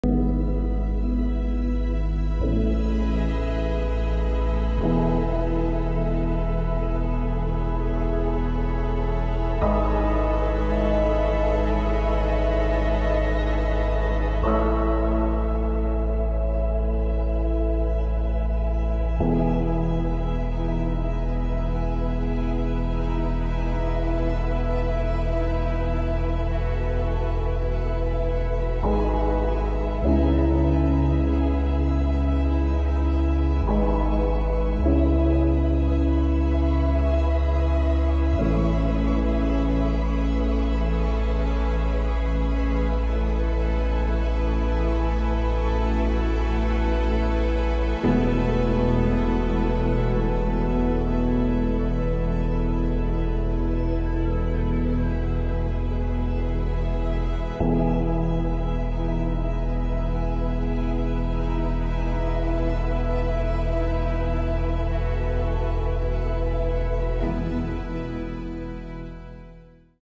HO and H0e Model Trams and Trains
The third collection of photos that shows some of my railroad modelling activities from 2016.